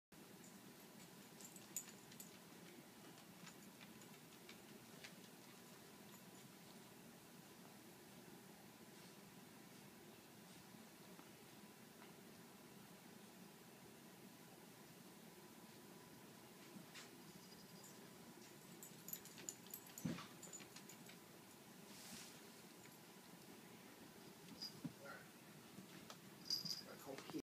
Saint Bernard puppy plays with cat
Baby Jasper plays with our Shooter Jennings cat